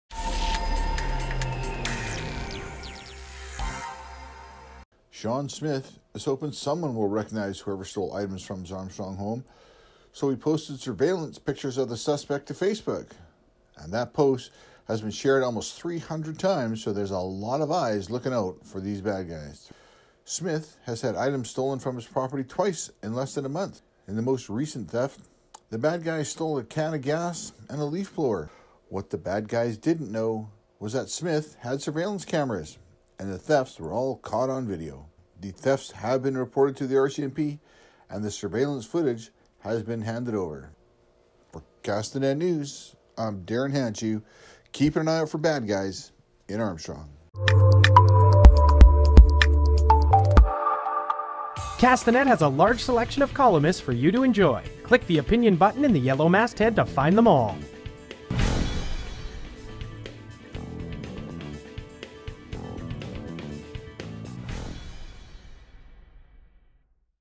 Caught on camera
Suspected Armstrong thief has been caught on camera